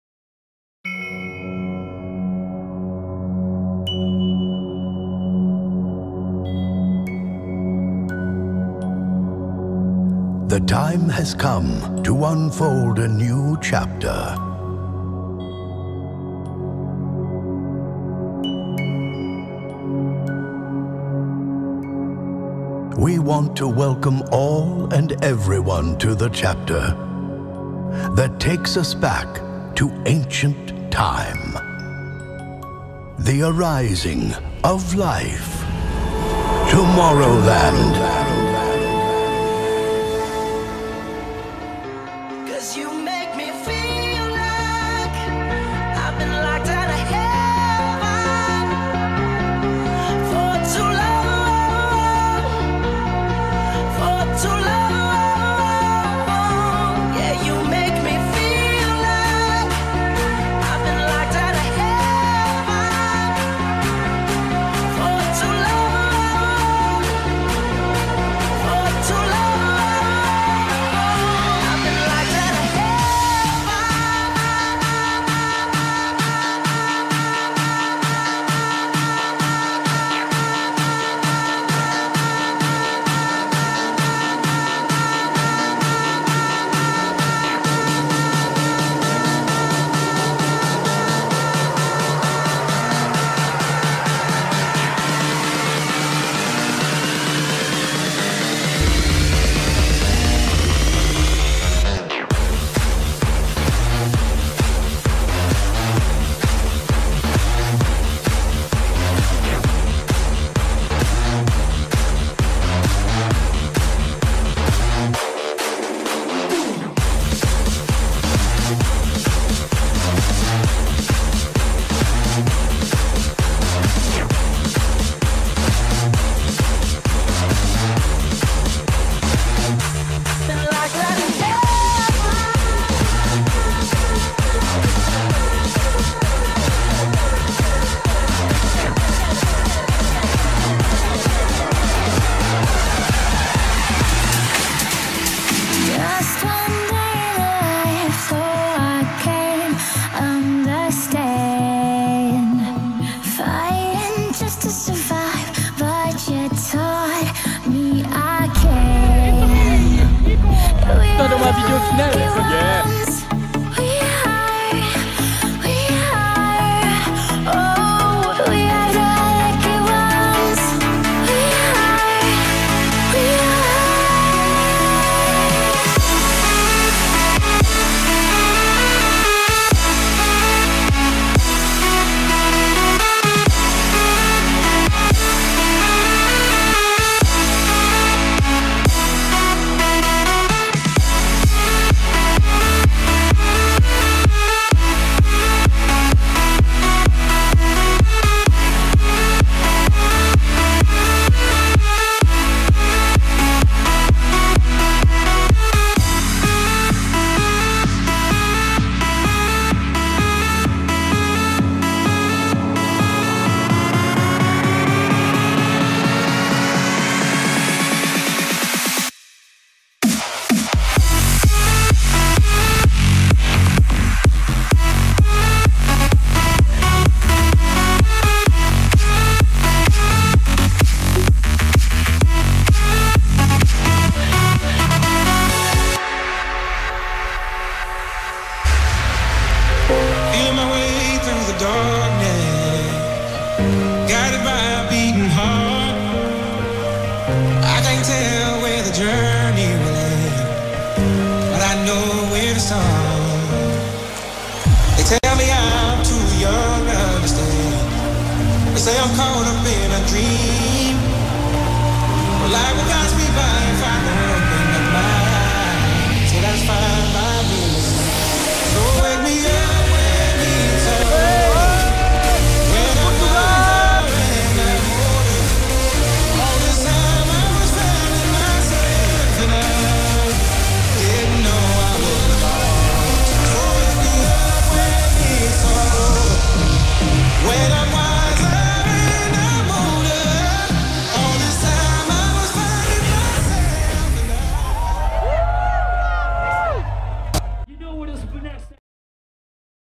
Tomorrowland - The Arising of Life 2013 Aftermovie
This is our journey to the most amazing festival in the World!!
It was filmed with my Gopro Hero 3